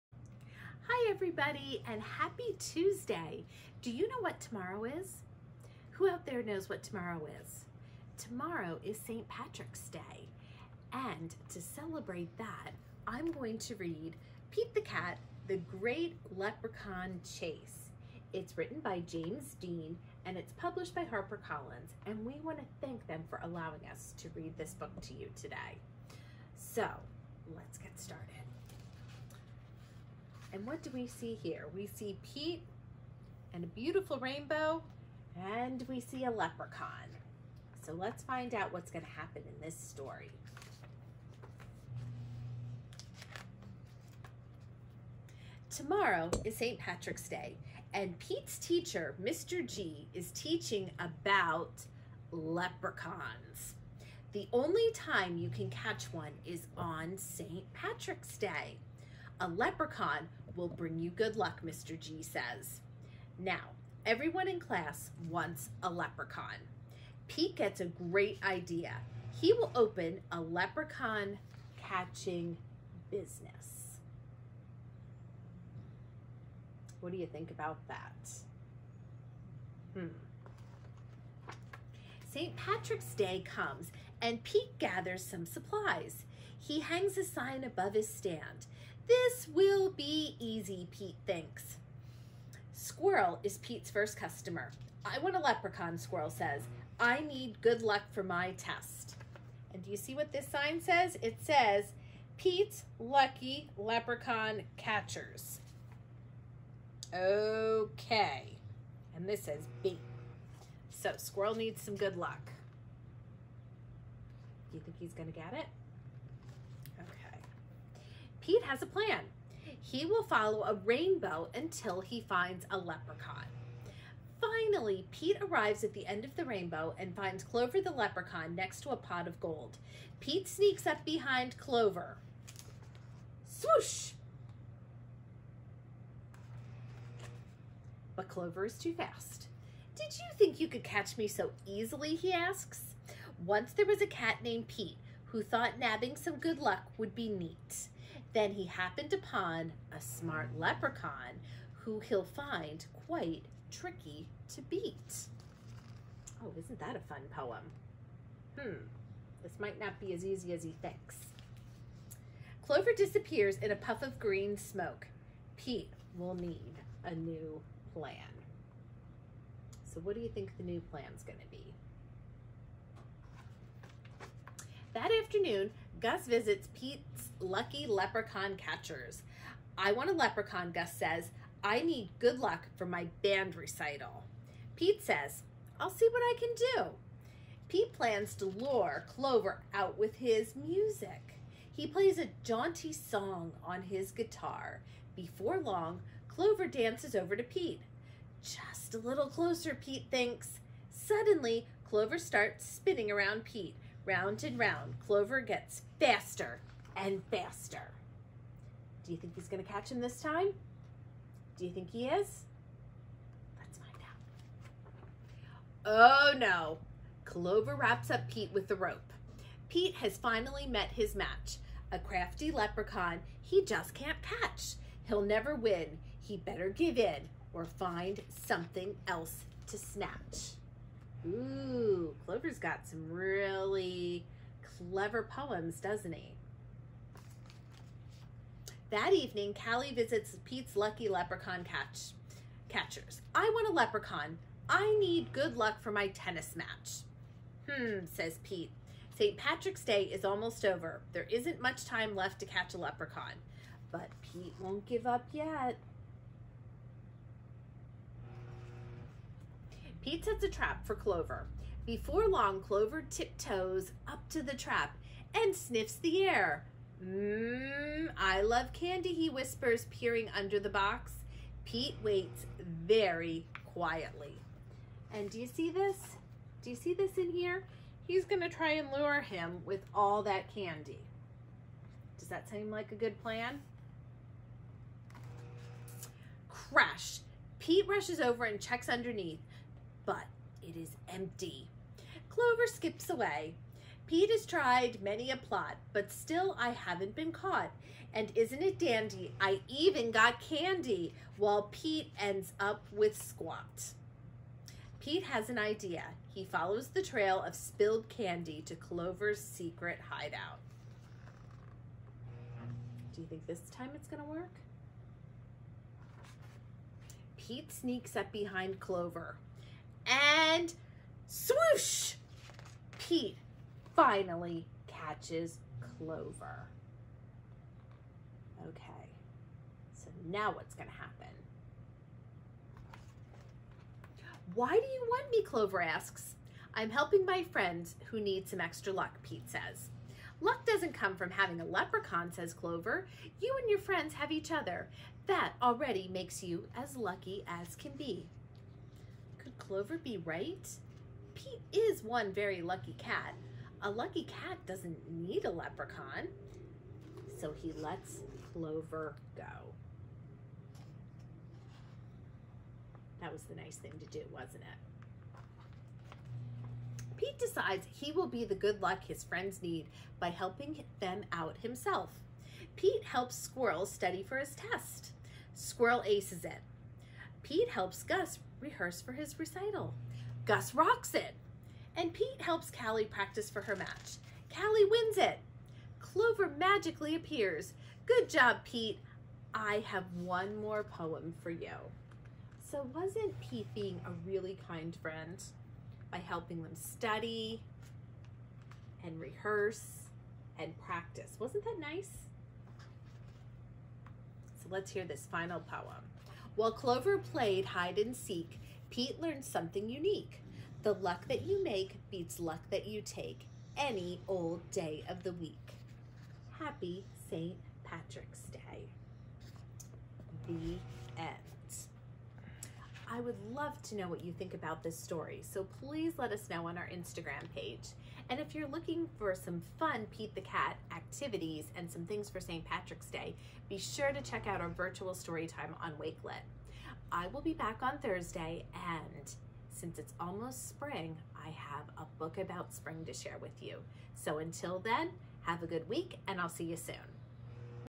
Story Time #85: Pete the Cat and the Great Leprechaun Chase by Kimberly & James Dean
Happy Tuesday everyone and good afternoon! We hope you had a great day at school. Thank you for joining us for RPL virtual story time, every Tuesday & Thursday at 3:30PM. Today's story is Pete the Cat and the Great Leprechaun Chase written and illustrated by Kimberly & James Dean.

Stop by the Reading Public Library, Main to check out our display of all the books we are reading this month to follow along.

Remember to download READSquared and choose the Reading Public Library in Reading, PA to add this program to earn badges and prizes.